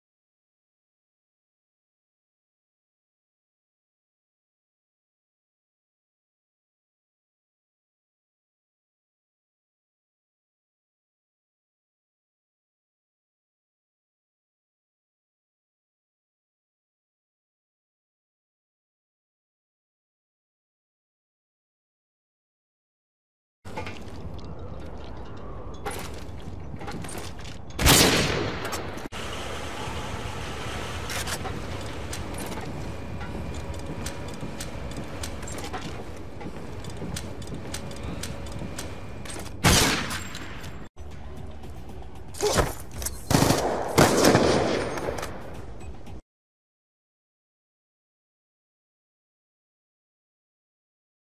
Chrome's Challenge Shot #1 (Watch Super Easy $10 Prize)
be ready to pause at the end!! Messed up rendering thing sorry and heres the link to that 30 second ladder YOU DO NOT HAVE TO HIT THE SHOT JUST BE CREATIVE AND/OR COMPLEX Go for it!!! ask questions i will reply assap